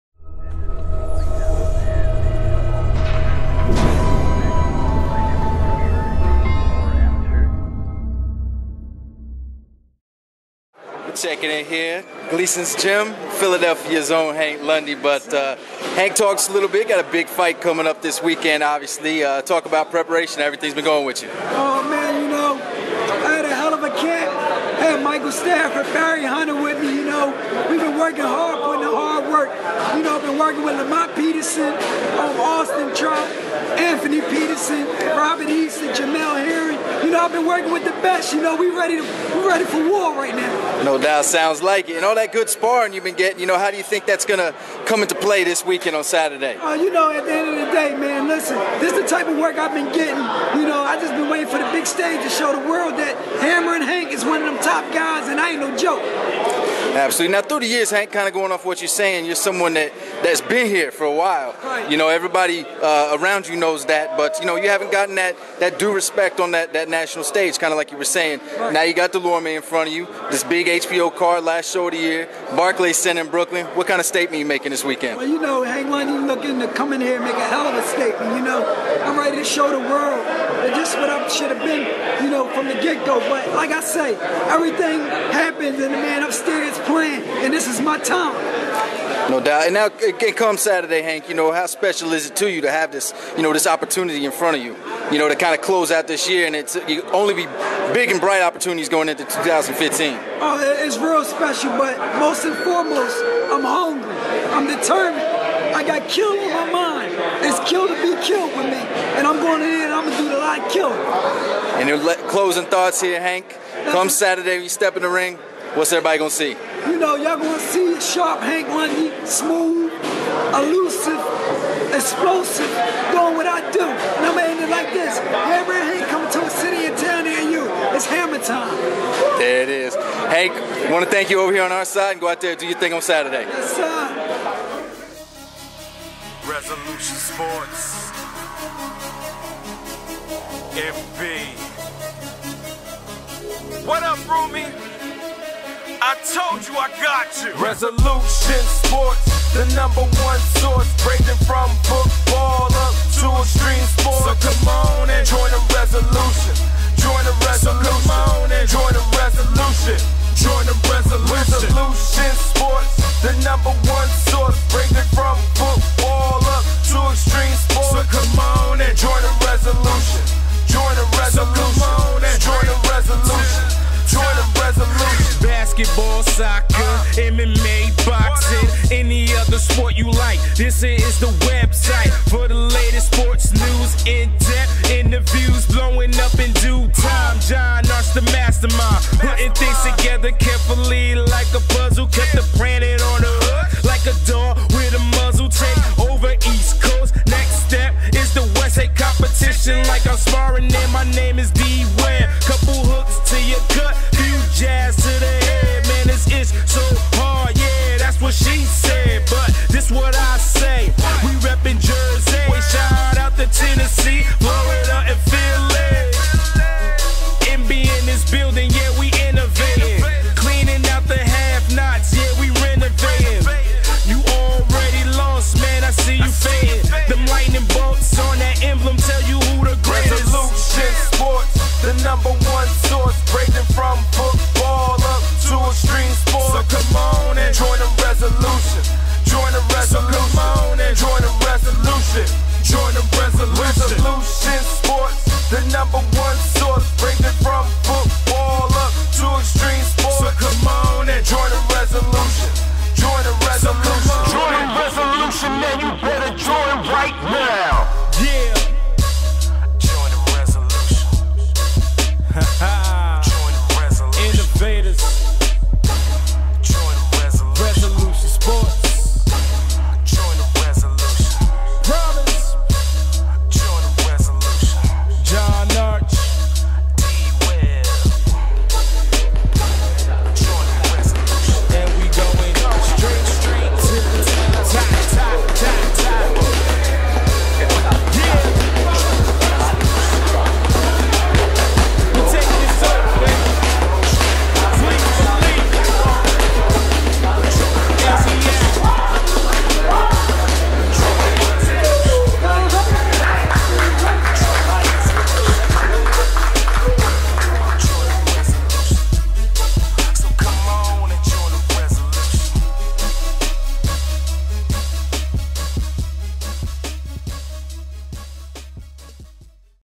Hank Lundy Media Workout
Highlights from a media workout by Hank Lundy (25-3-1, 12 KOs) in preparation for his fight with Thomas Dulorme (21-1, 14 KOs) at Barclays Center. This footage was shot at Gleason's Gym in Brooklyn.